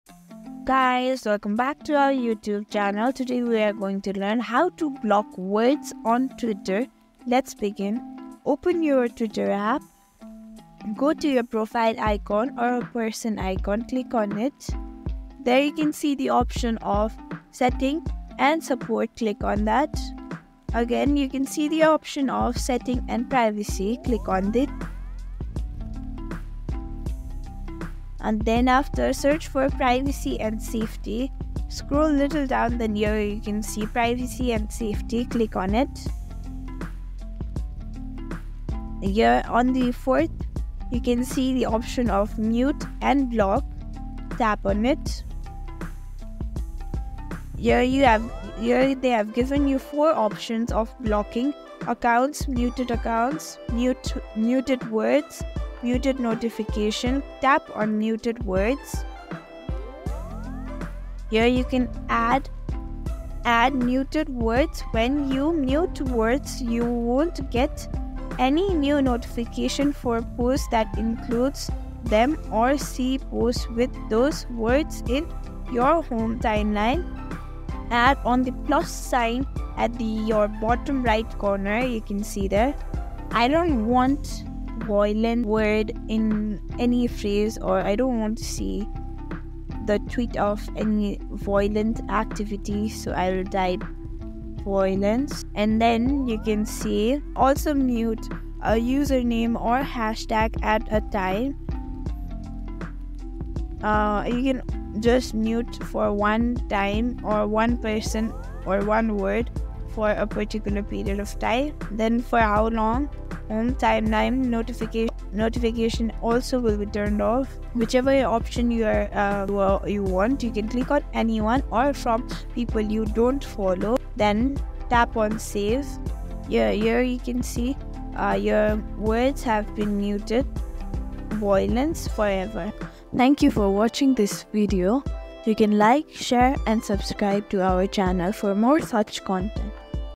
How to Block Words on X or Twitter 2024
Want to filter out unwanted words or phrases on X or Twitter? Our tutorial for 2024 walks you through the steps to block specific words, ensuring a more tailored and enjoyable online experience. Whether you're avoiding spoilers, sensitive topics, or just curating your feed, we'll show you how to take control of your content. Watch now and customize your X or Twitter experience today!